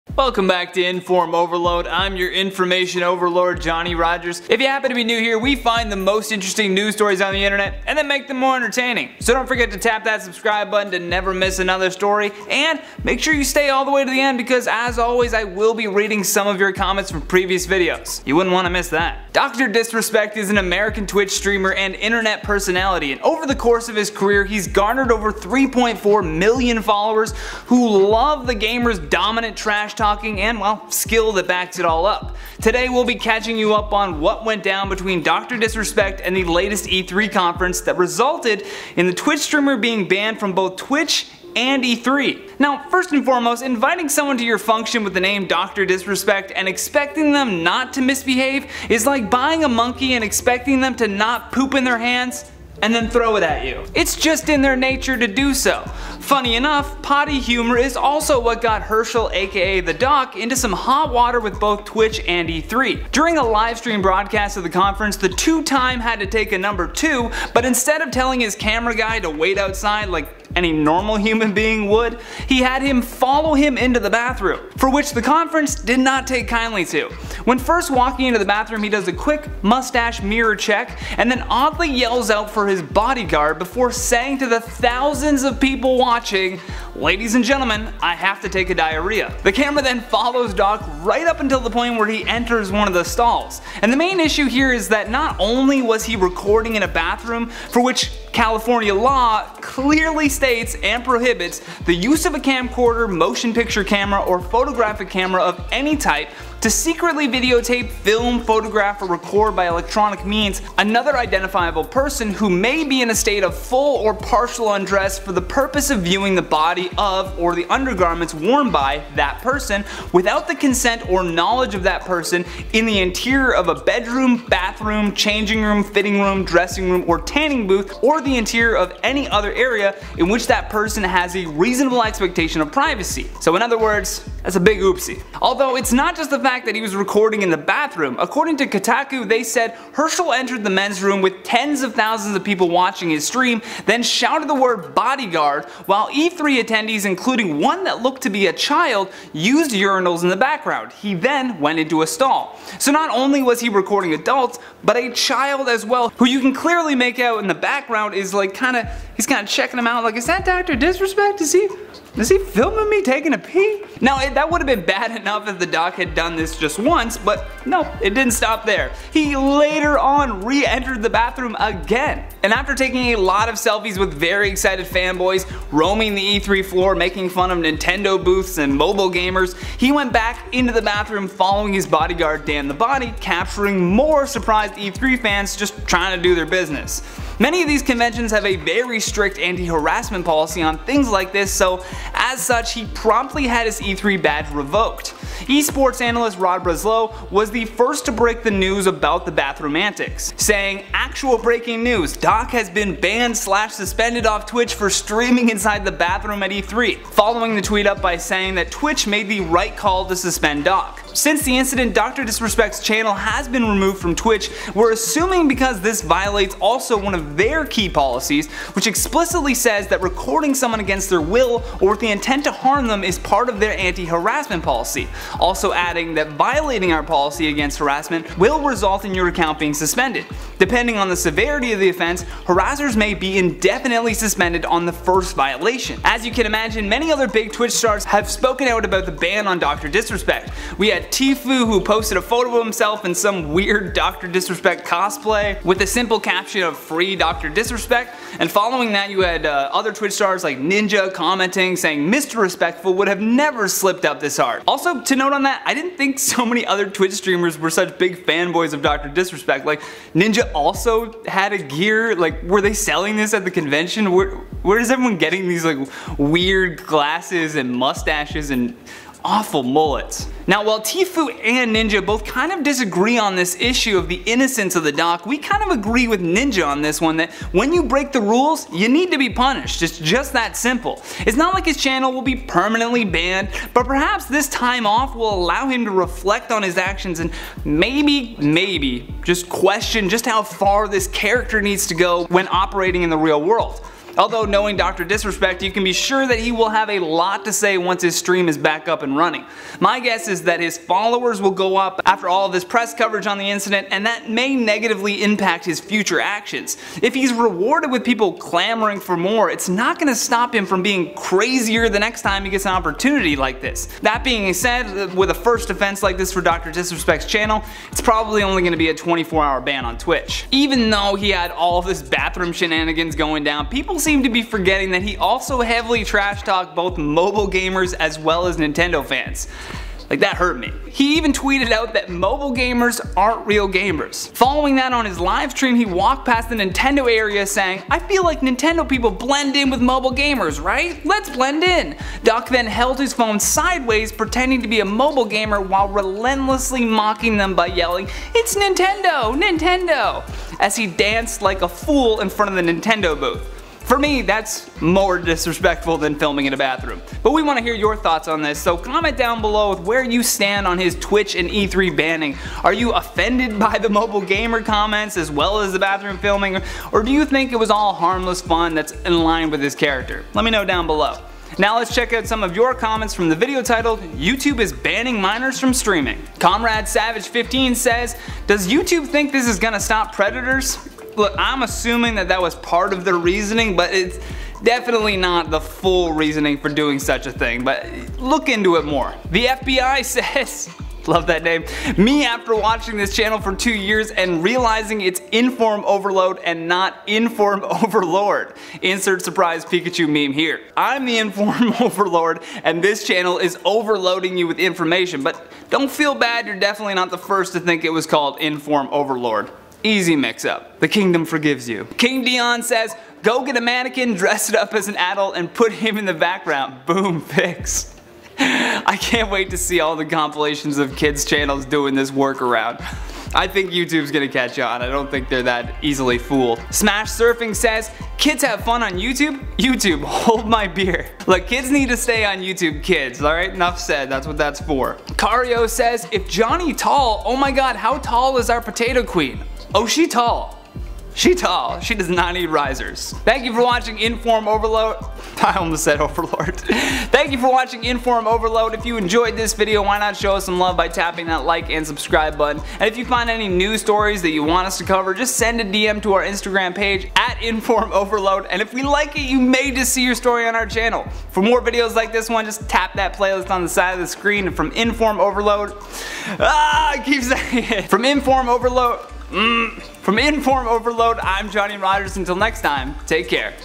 Dr Disrespect Banned From Twitch
Dr Disrespect Has been banned from twitch after he did a live stream in a bathroom at the E3 conference. And apparently his e3 badge has also been revoked.